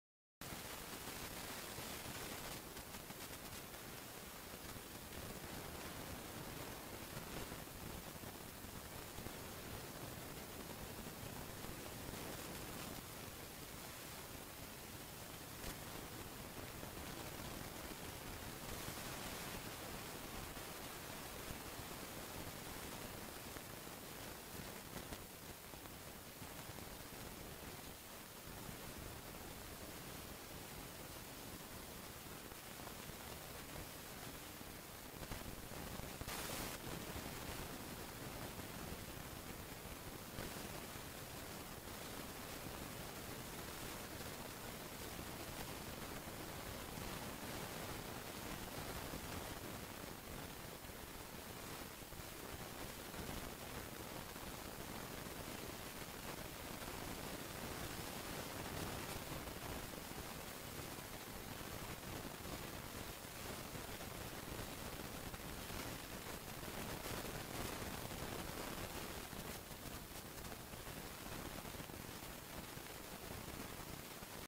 ม้าซ้อมสนามโคราช วันพฤหัสที่ 20-12-61 ปทุมวดี-เลิศชัย
ม้า ปทุมวดี  จ๊อกกี้ เลิศชัย (ตัวนอก)
(ซ้อมประกบคู่ให้ลูกม้าใหม่)
ม้าซ้อมสนามโคราช วันพฤหัสที่ 20-12-61